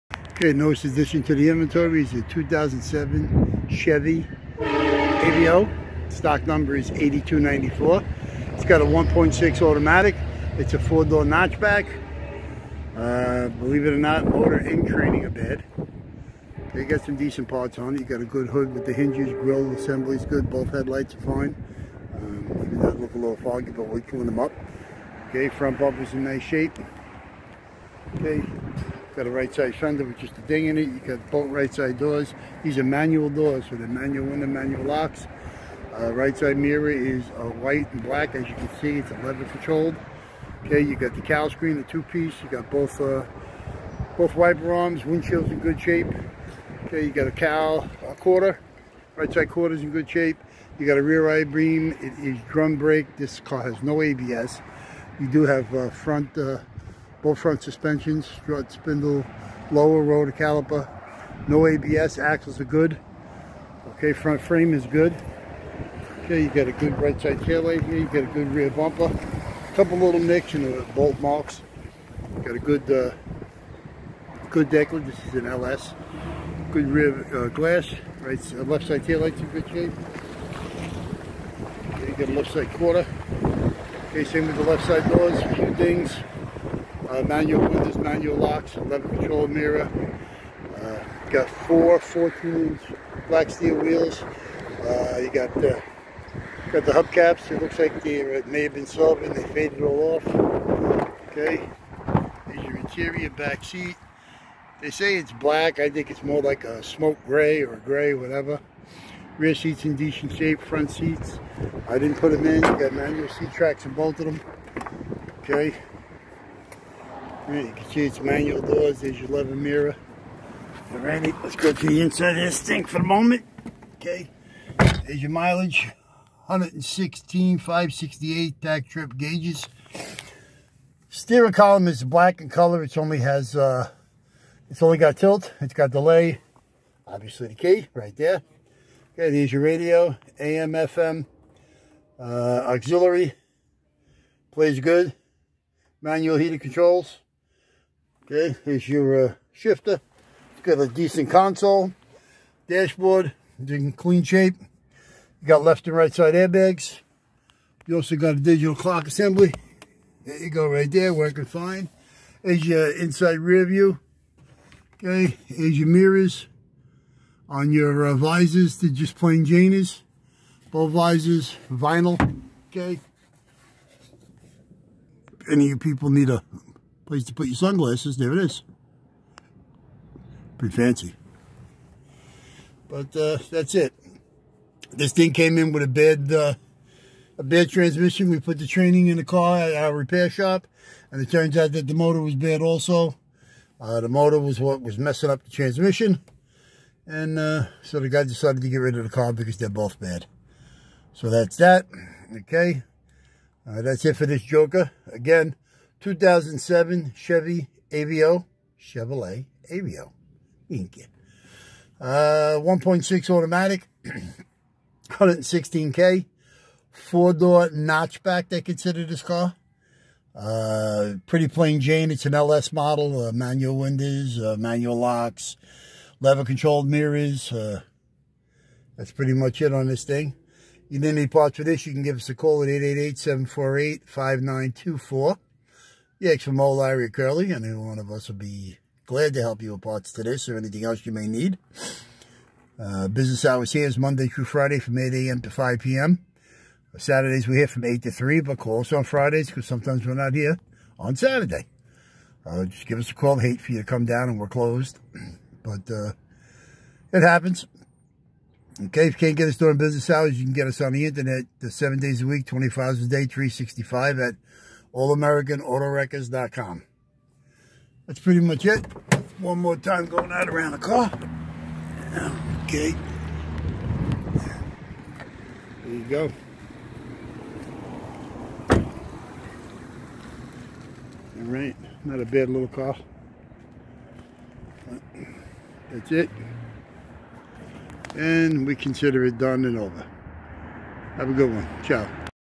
All American Auto Wreckers is Dismantling thus 2007 Chevy Aveo 4dr notchback 1.6 At Stock # 8294
This 2007 Aveo LS 4dr Notchback came in running and driving with a bad engine and a bad automatic transmission also.  Body is in fair shape with a few dents around. This Aveo was a plane model with manual doors and locks. Also lever control mirrors painted white. This 2007 Aveo came in with 14 inch black steel wheels and hub caps. The 1.6 engine was bad as stated but we do have all the parts from the motor. It did runs but knocked and the transmission slipped. Exterior Paint Code is-11U-White. The Interior trim Code-81I-Gray Cloth. And the Build Date is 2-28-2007. So if Moe-Larry or Curly can help you with any parts from this 2007 Chevy Aveo 4dr notch back call us Toll-Free at .